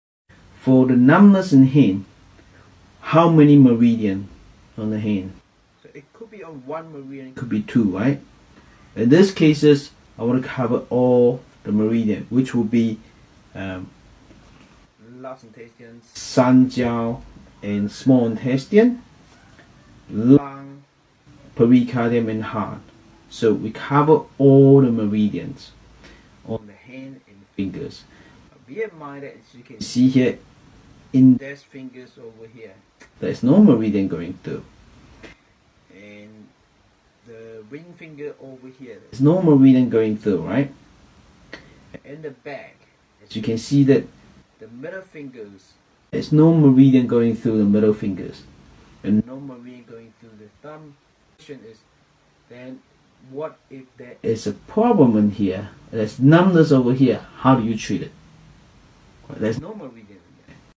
Penetrating Deeper: Exploring the Wonders of New Balance Method Acupuncture for Meridian Health and
Penetrating Deeper: Exploring the Wonders of New Balance Method Acupuncture for Meridian Health and Alleviation of Hand Numbness In the realm of traditional Chinese medicine, the New Balance Method Acupuncture carries a significnt role, bridging a vital connection between the human body and nature. It is not merely a series of acupuncture points, but an exploration in the intricate dimensions of life. It is within this context that the potency of this method truly shines, specifically when dealing with meridian imbalances and hand numbness. As we delve deeper into the nexus of New Balance Method Acupuncture, the meridian on hand, and the treatment of hand numbness, we transcend beyond the superficial understanding of this ancient practice and open a new pathway to the understanding of holistic health. The meridian element, extensively researched since ancient times, connects us to the vital energy flow within our bodies. The use of the New Balance Method Acupuncture amplifies the efficacy of this connection and bridges the gap between the physical and spiritual dimensions. Where to learn ?